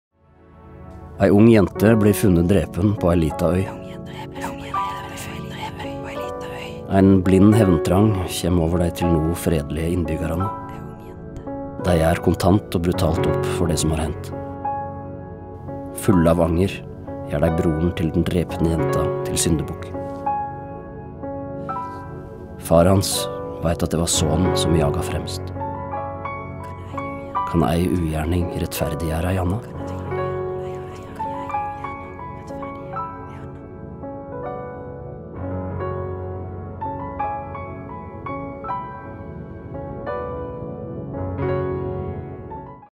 Kimen av Tarjei Vesaas
Kva ansvar har den enkelte for ei ugjerning alle var med på?

Ei ung jente er drepen. – Kven har gjort det? Nyheita om drapet slår ned som eit lyn i det tilsynelatande uskyldige øysamfunnet. Den framande Andreas Vest blir raskt mistenkt, og det utviklar seg til ei nådelaus jakt. Øybuarane tek ei kollektiv avgjerd om å ta lova i eigne hender.

Kimen har premiere 19. januar 2018 på Det Norske Teatret. Les meir